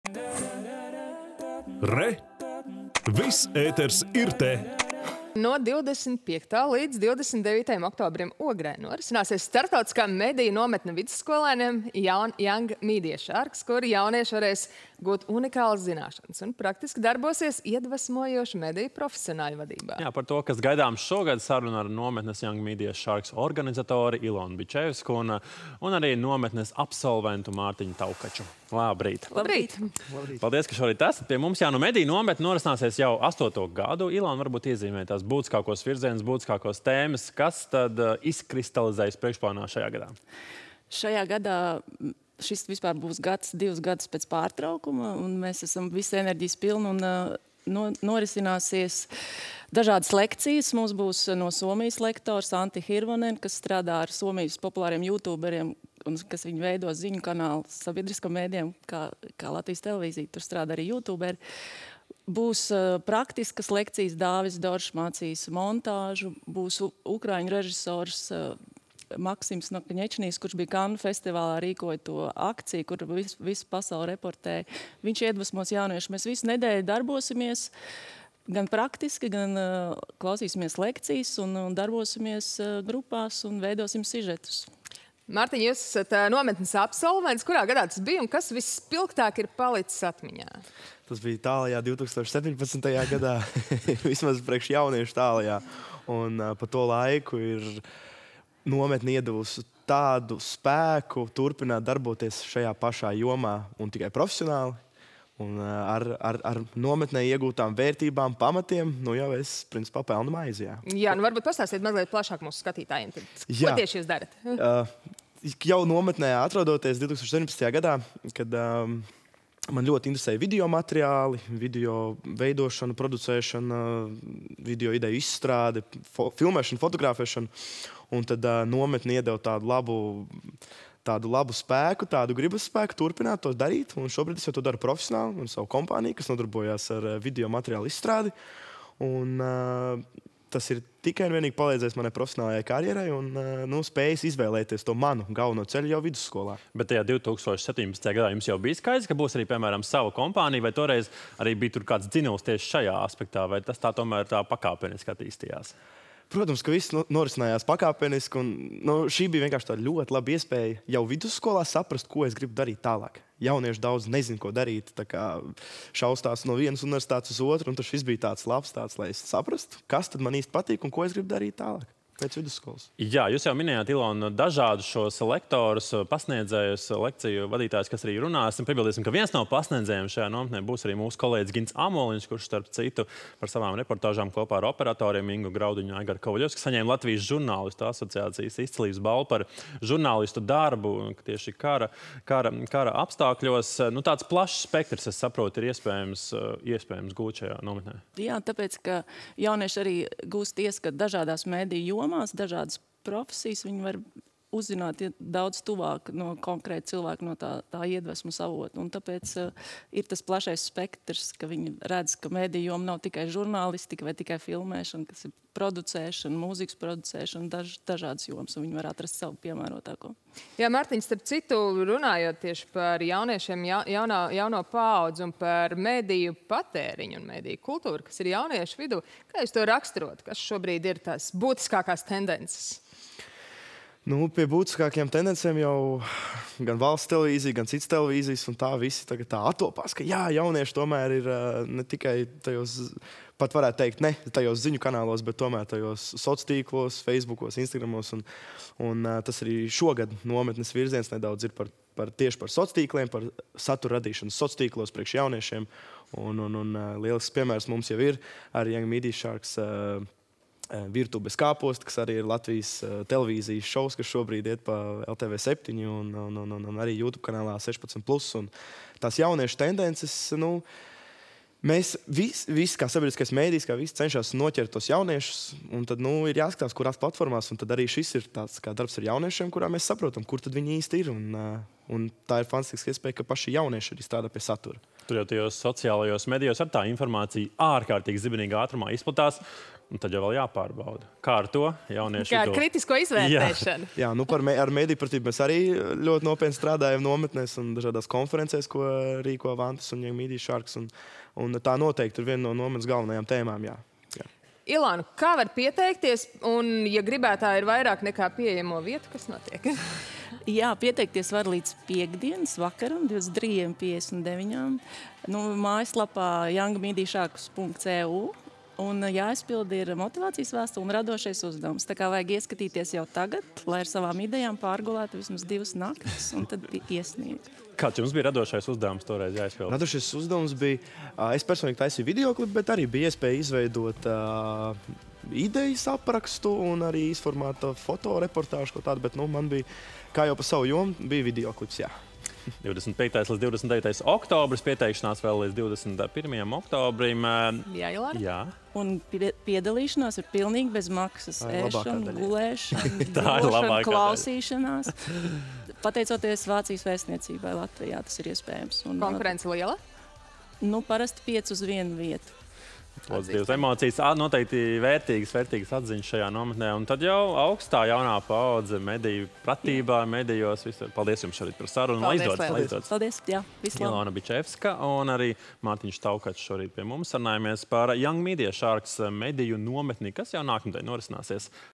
Izglītojošā jauniešu nometne “Young Media Sharks” notiks jau astoto gadu
No 25. līdz 29. oktobrim Ogrē norisināsies starptautiskā mediju nometne vidusskolēniem “Young Media Sharks”, kur jaunieši varēs gūt unikālas zināšanas un praktiski darbosies iedvesmojošu mediju profesionāļu vadībā.
    Par to, kas gaidāms šogad – saruna ar nometnes “Young Media Sharks” organizatori Ilonu Bičevsku un arī ar nometnes absolventu Mārtiņu Taukaču.